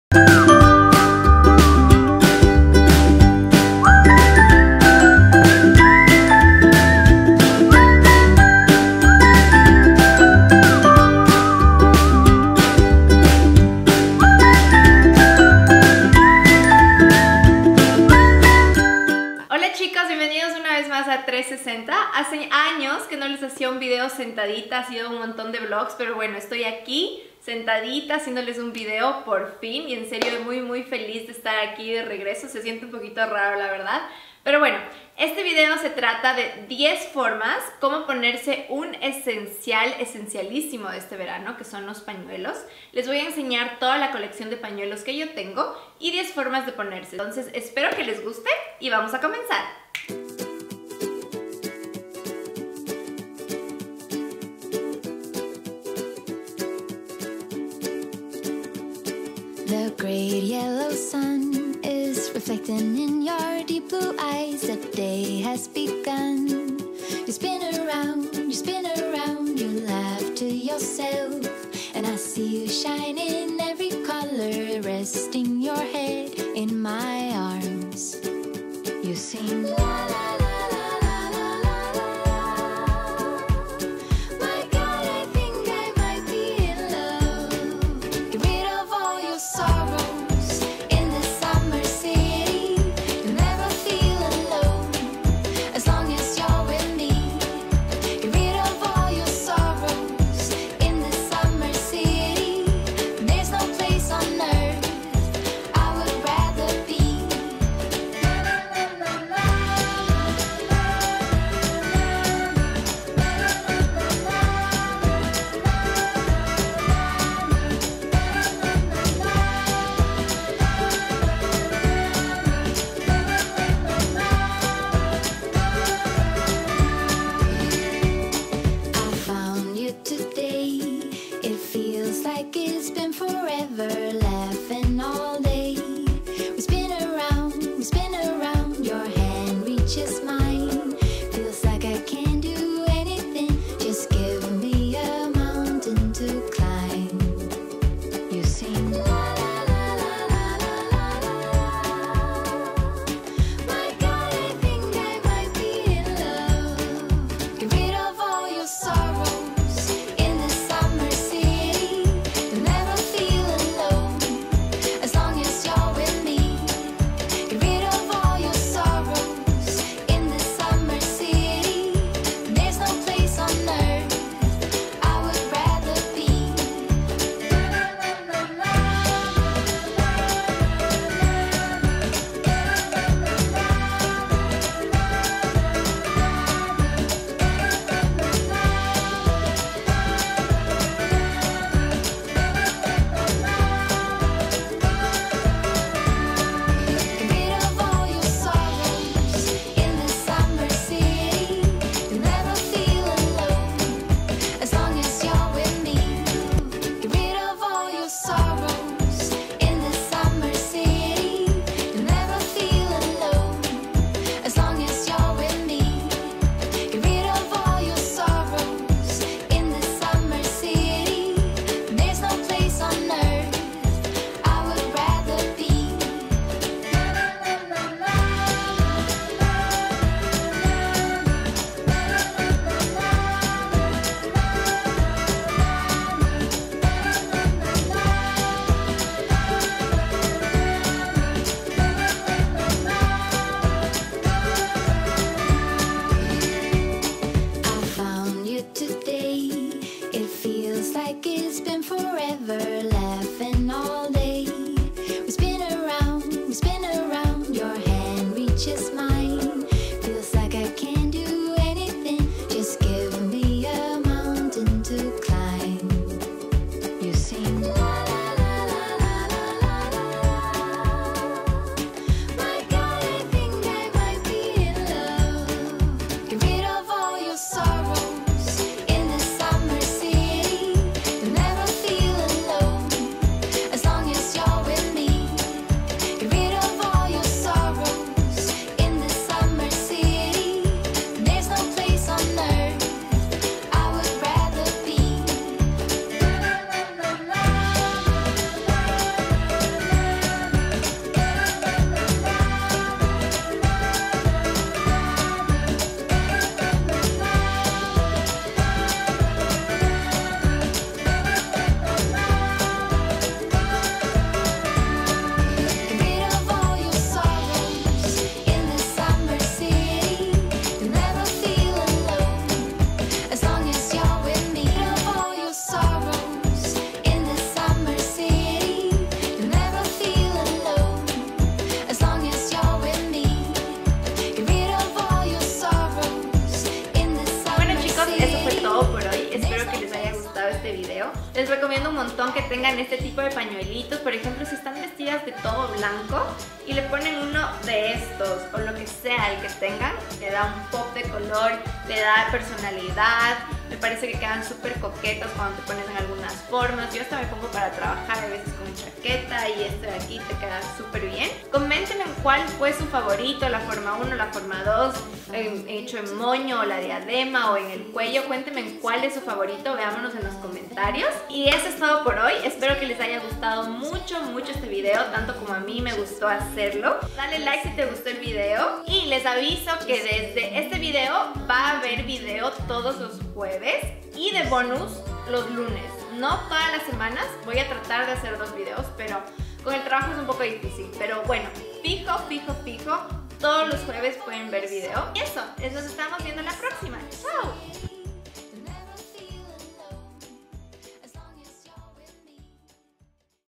10 Ways to wear a Bandana ♥ SUMMER EDITION // Sofia 360
Chic@s en este video les presento 10 ideas para ponernos los pañuelos, un accesorio super versátil y esencial para este verano! Es una manera de animar nuestro estilo y util para ocultar un 'bad hair day'. A mi encanta usarlo en la playa, cuando hay mucha brisa :) En resumen, el pañuelo es un básico y esencial del que podemos sacar mucho provecho.

Espero que les haya gustado! Nos vemos la próxima semana!
VIDEO NUEVO TODOS LOS JUEVES

BESOS a todos Sofia 💋💋💋